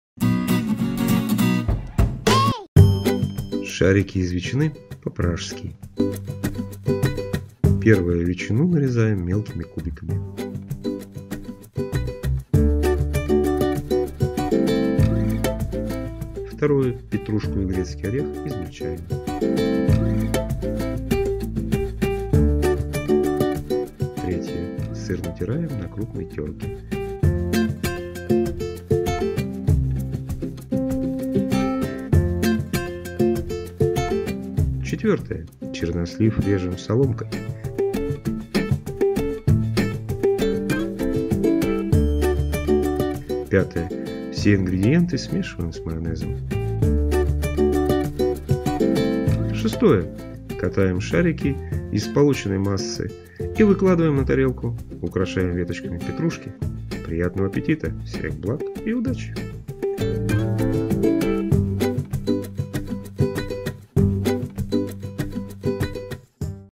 Закуска "Шарики из ветчины по-пражски" на новогодний стол
Простой рецепт закуски на ваш новогодний стол.